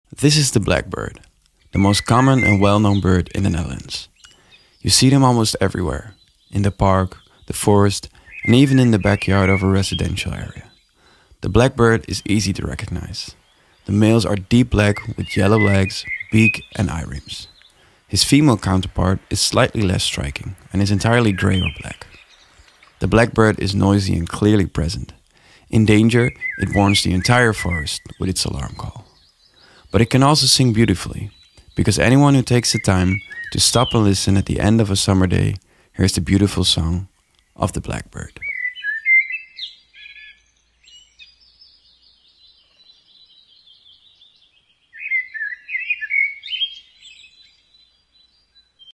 Let's meet the blackbird!
Let me introduce you to the blackbird. This most common bird in the Netherlands is a bird that you can encounter everywhere.